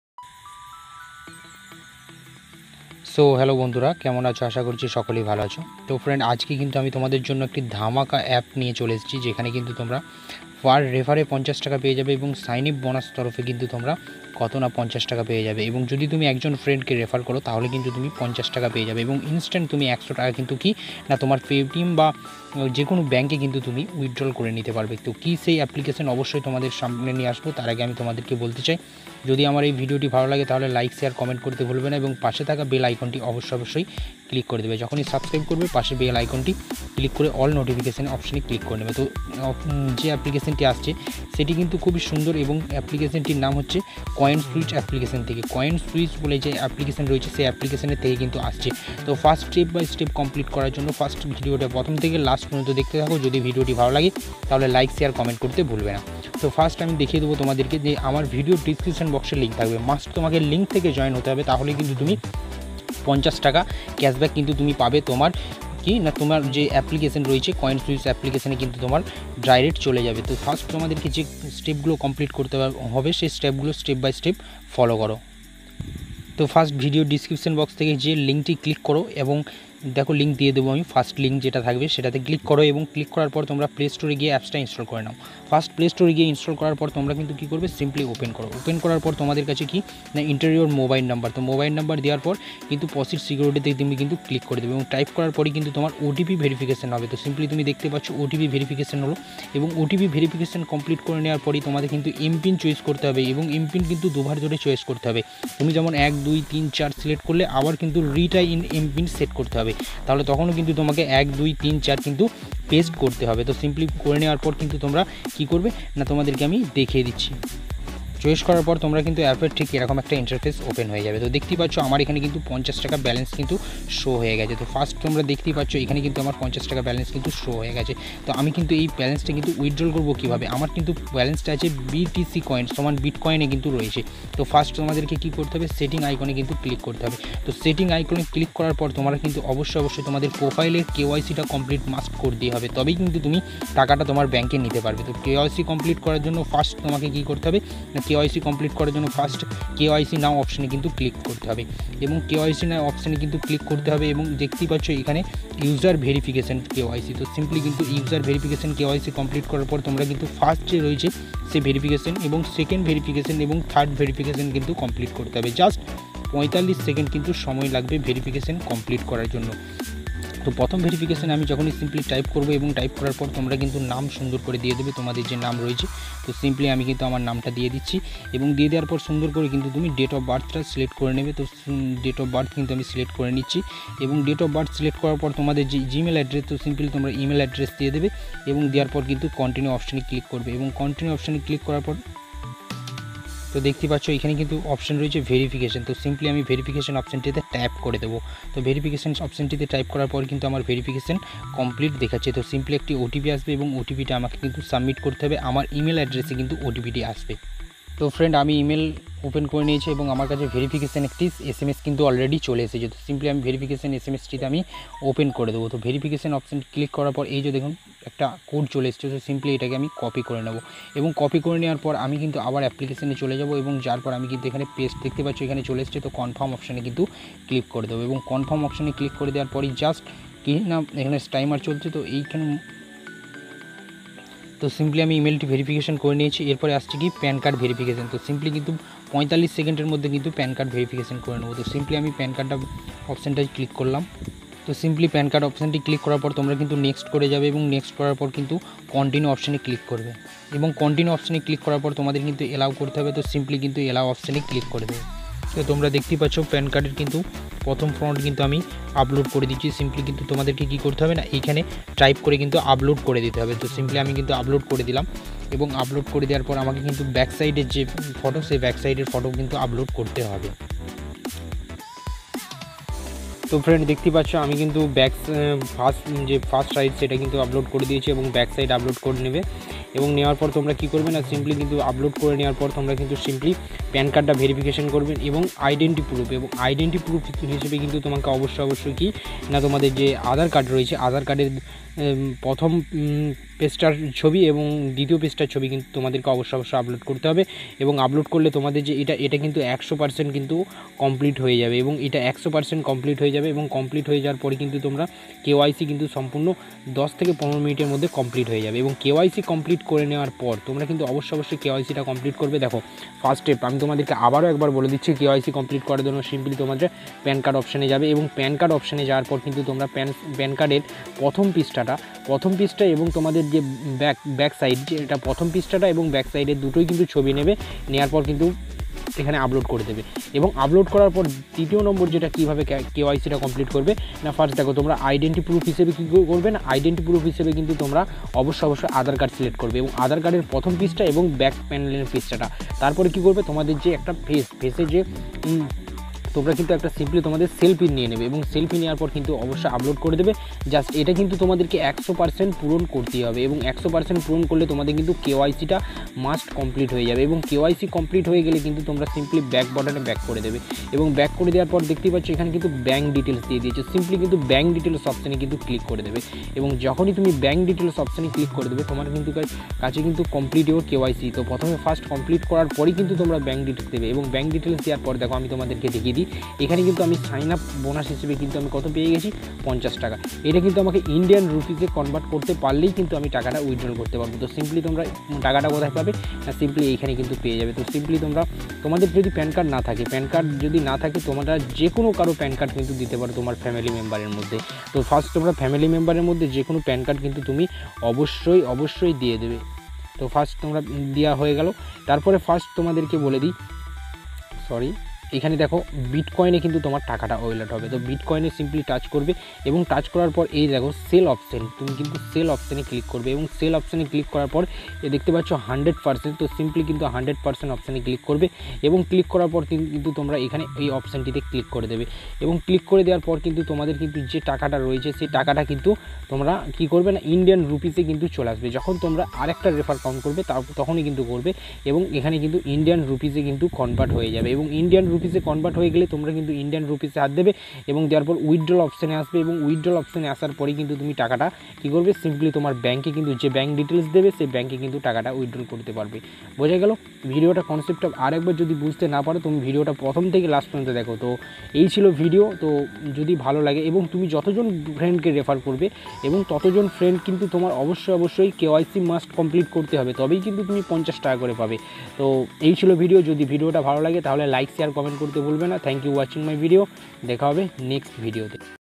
Coinswitch App in Bangali ।। 50+50+50+50 Direct Bank।। Paytm Bank Withdrawal করুন। ।
🙏🙏🙏Welcome🙏🙏🙏

 Hi Friends..

About this video :- তুমি অ্যাপ্লিকেশন টি ওপেন করলে। এবং ওপেন করার পর তোমাকে KYC কমপ্লিট করতে হবে । না হলে কিন্তু তুমি ব্যালেন্স কিন্তু পাবেনা। তোমার ওয়ালেটে এবং তুমি টাকাও কিন্তু ব্যাংকে ট্রান্সফার করতে পারবে না।